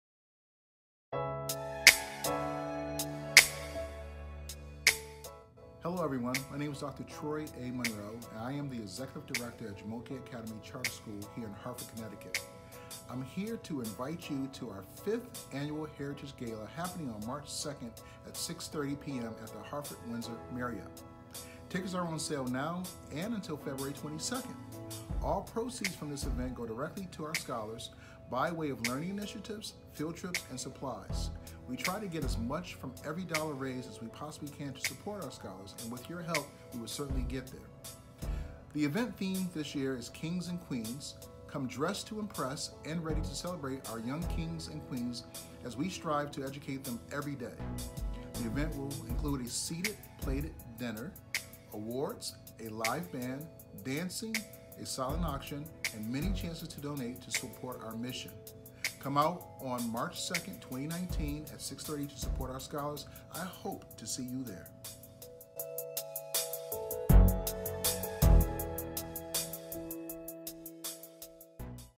5th Annual Heritage Gala
A message from our Executive Director about this year's Heritage Gala...